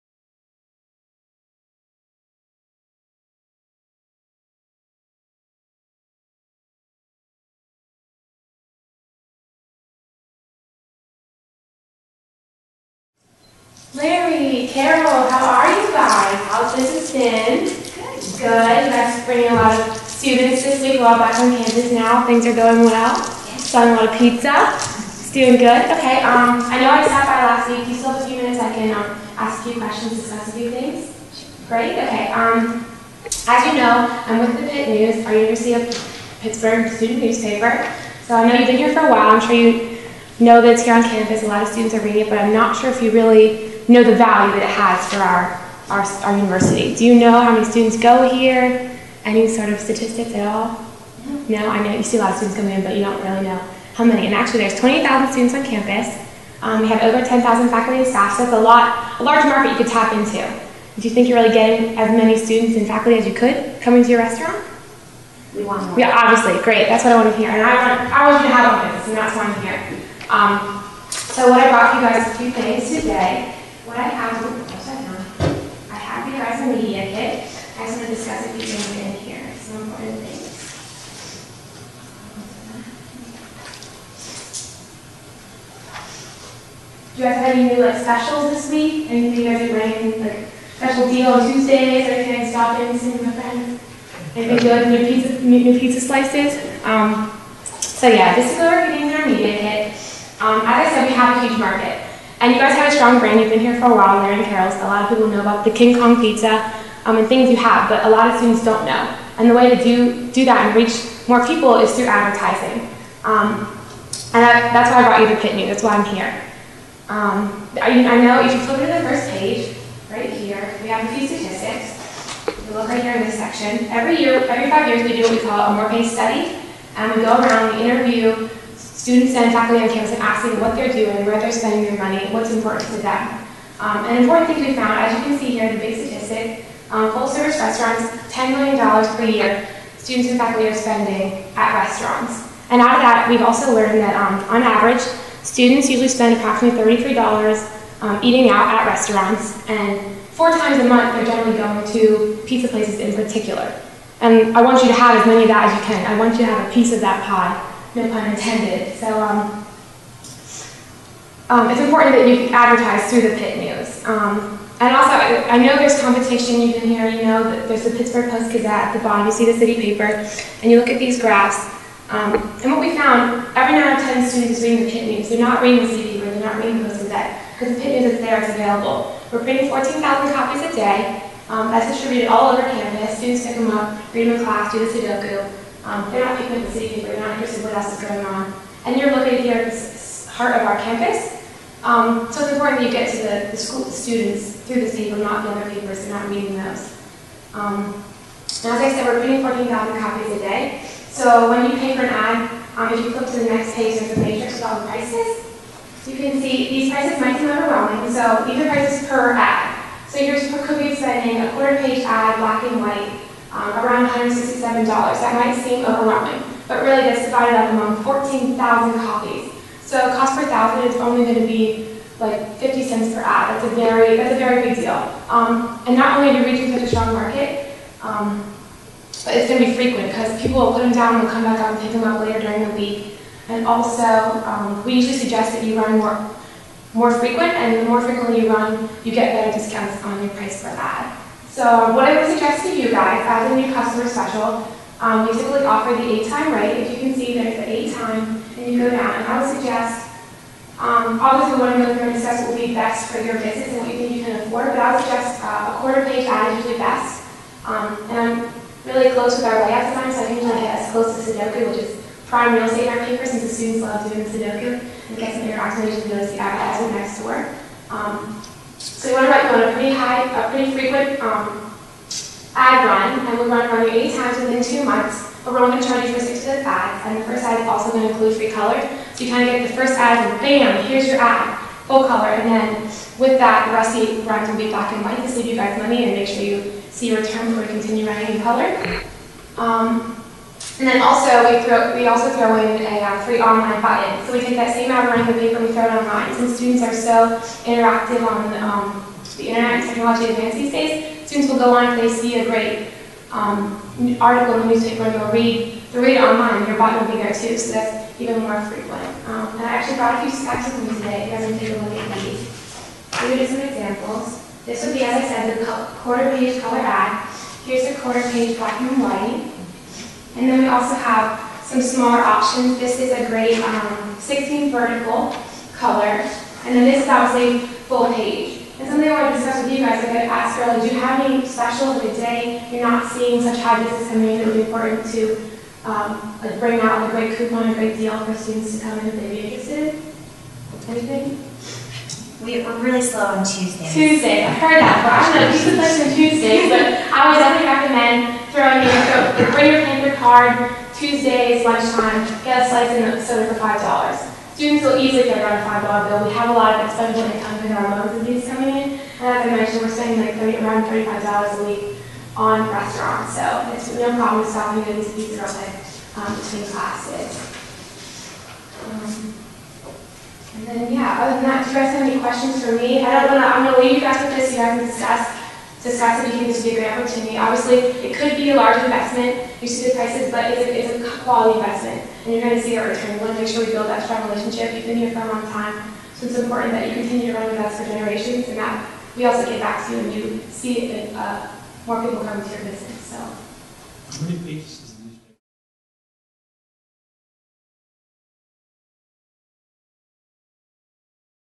2011 PBL Sales Presentation
1st place winner at 2011 FBLA-PBL National Leadership Conference - Orlando, FL